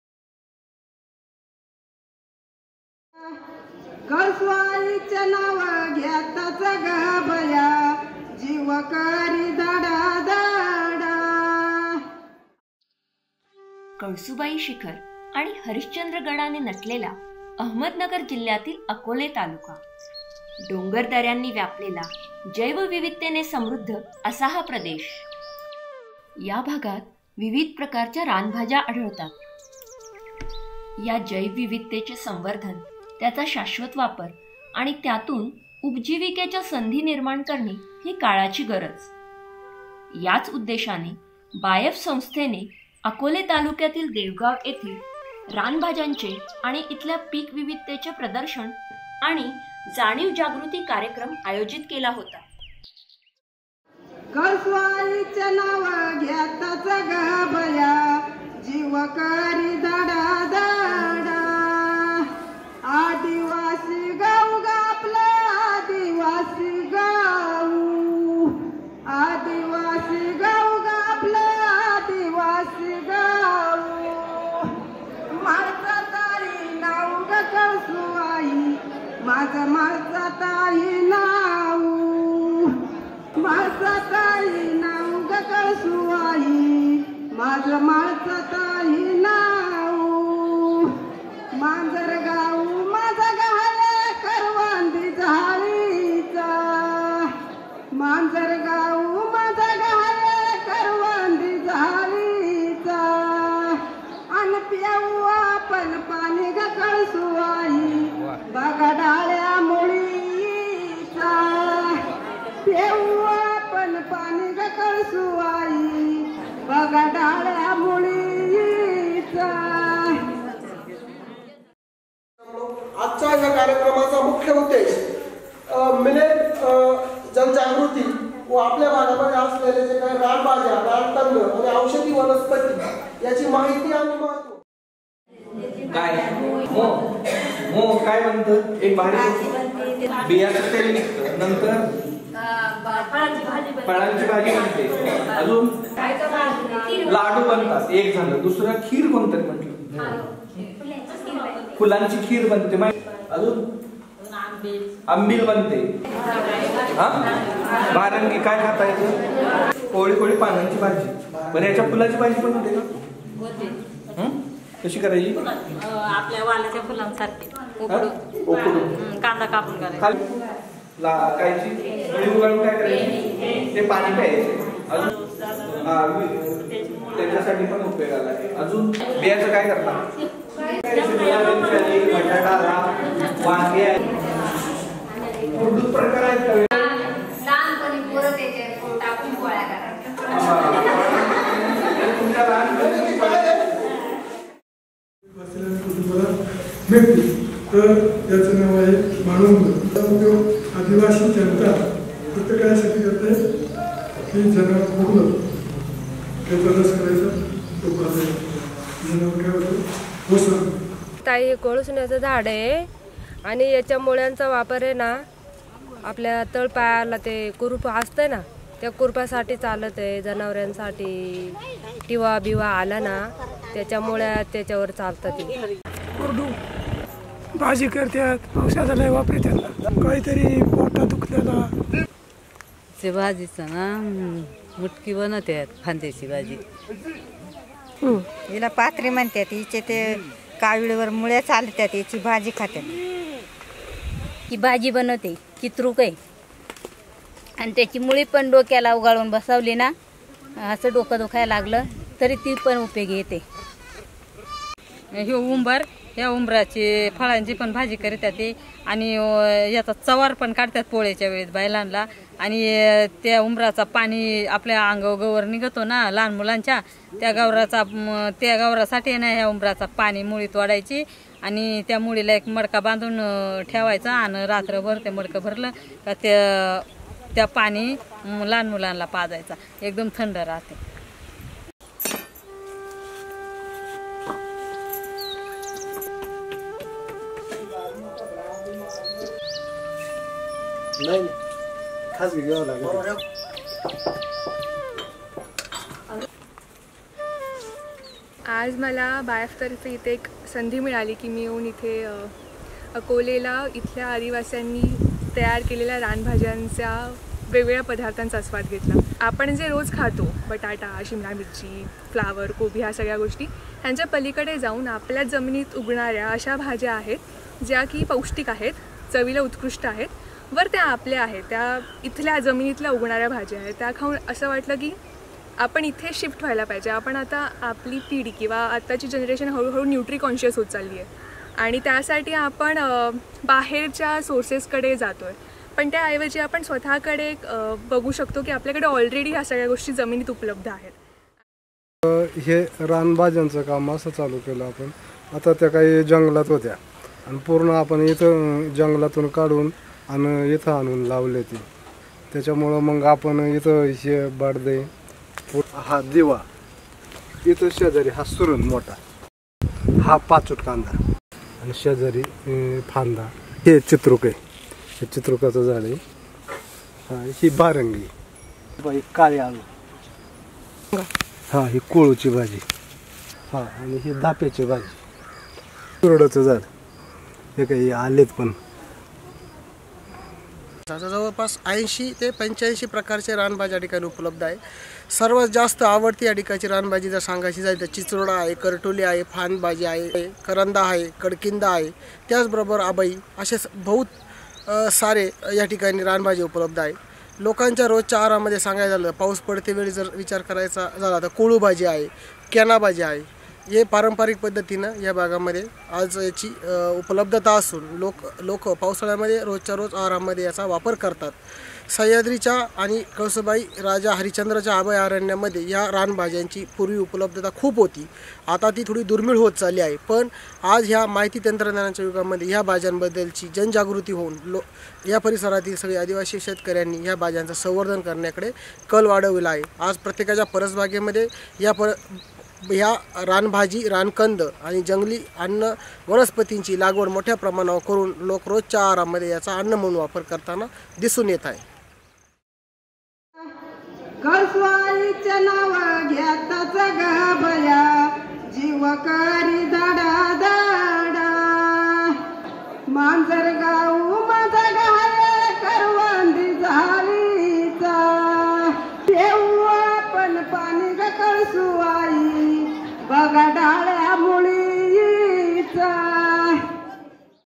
BAIF | Wild Vegetable & Seed Festival | 2023
Wild food festival organized at Devgaon Village of Akole,Ahmednagar District,Maharashtra on 11th Aug 2023 as a part of BAIF-Seed bank Project Akole supported by " Shabri Adivasi Vitta V vikas Mahamandal,Nashik"